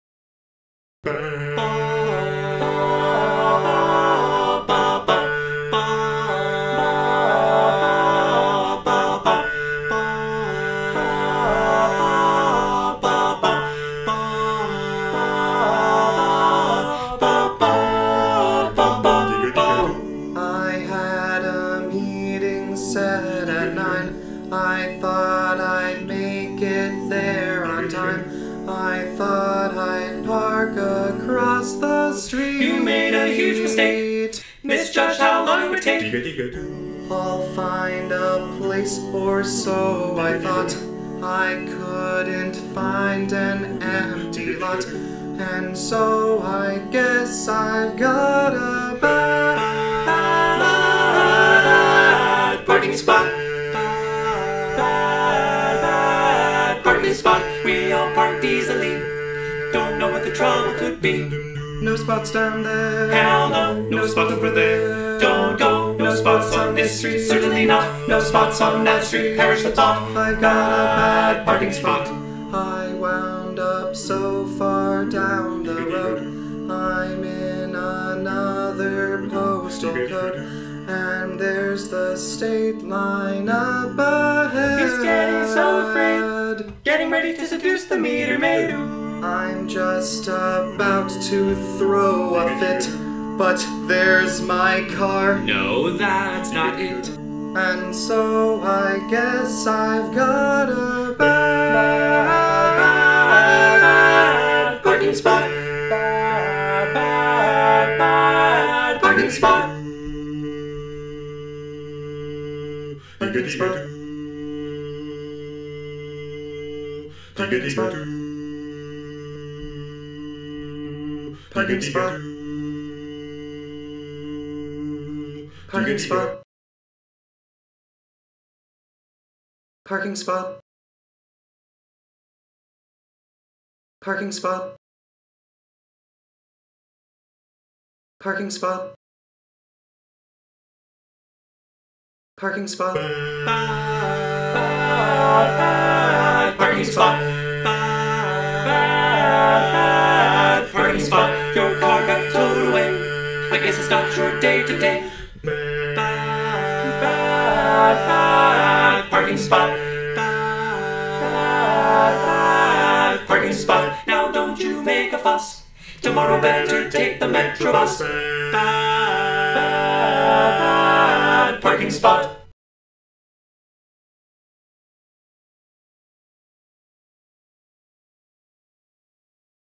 Bad Parking Spot
When life gives you lemons.

From our 2015 Spring Show: The Spring Olympics
Soloist: Jeremy Raven